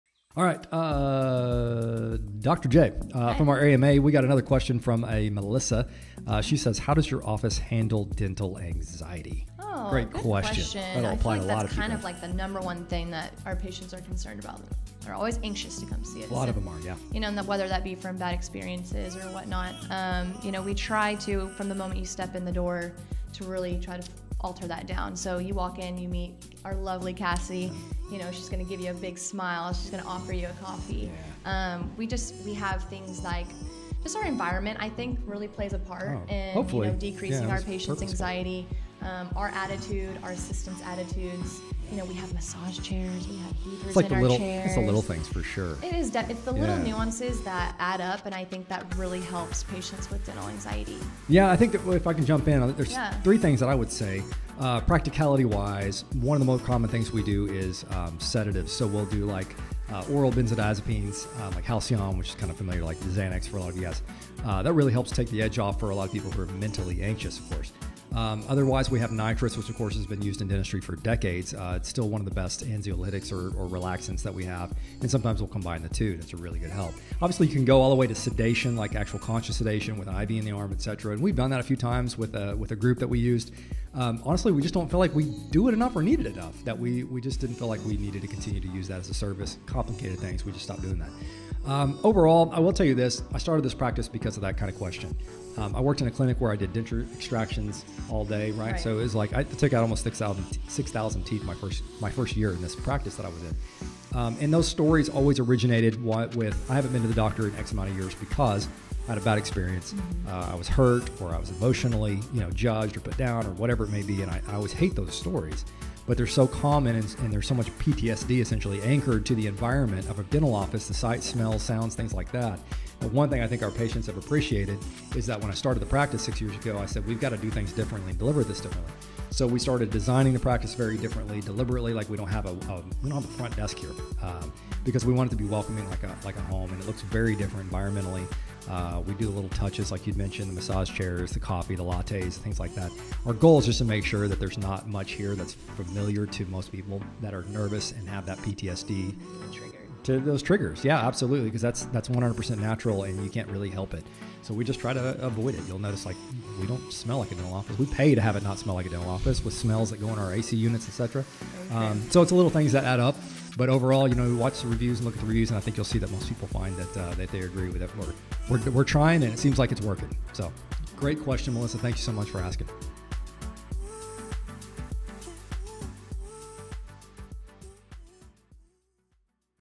Dental anxiety? Help!?! We got you.
Nervous? Dentistry, yikes! Yeah we understand. Here's how we adress it AND it works!